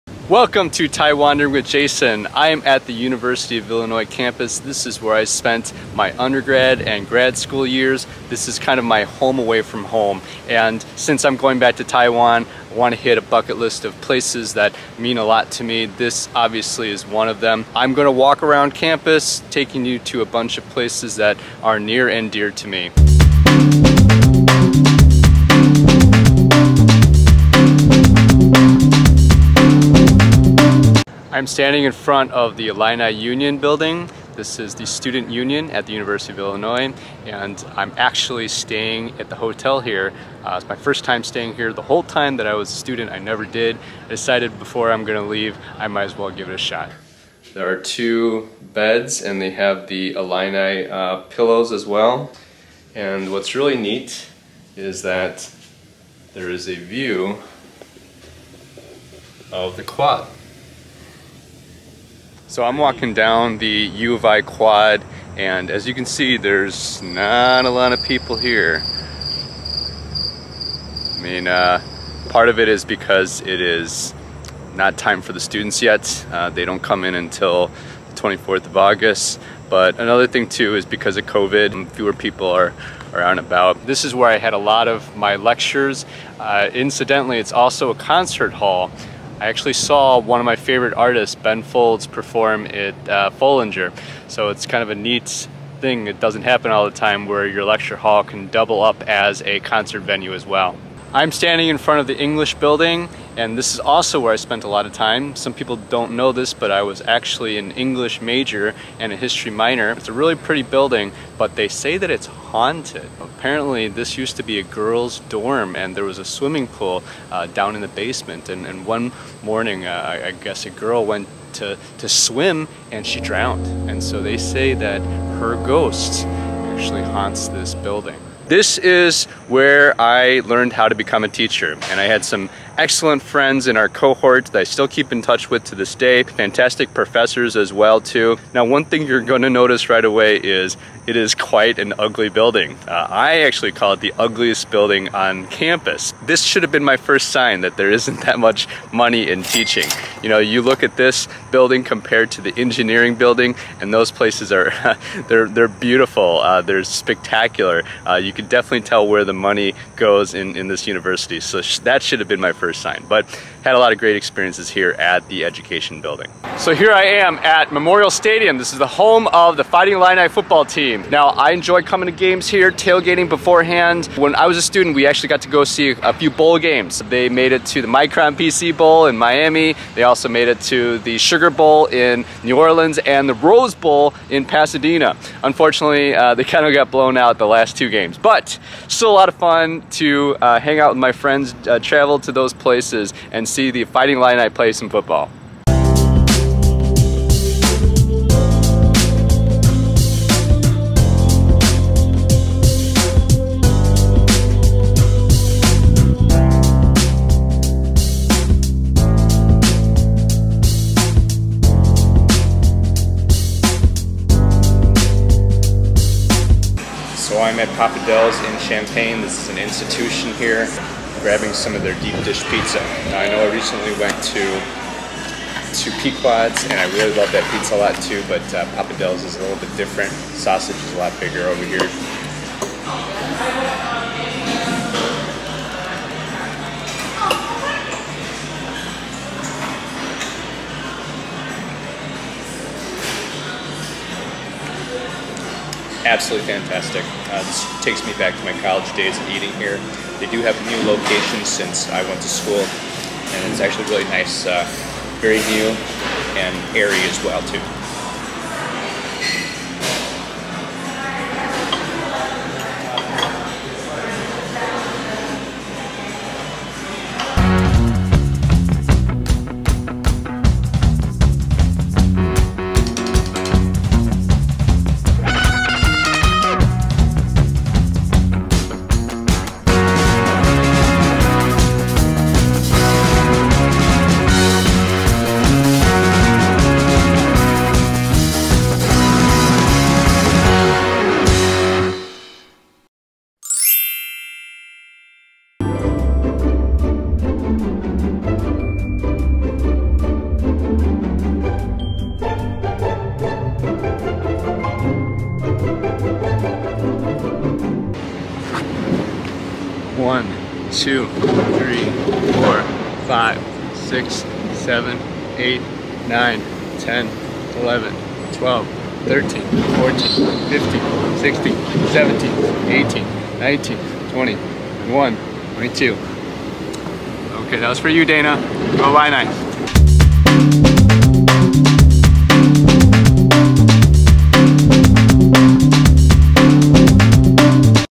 Heading back to the University of Illinois campus - Ep. 11
Going back to the University of Illinois campus before I head to Taiwan.